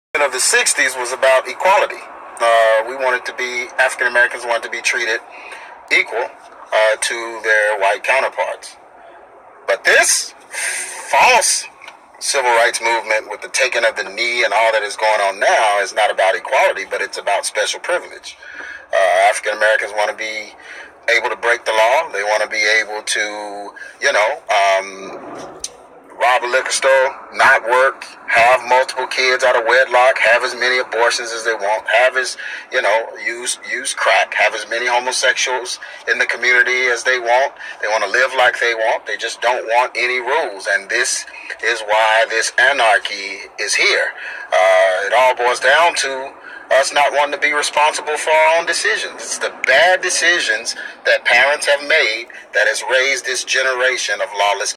The Truth About 2017 Racism
Pastor G. Craig Lewis of Ex Ministries breaks down today's racism.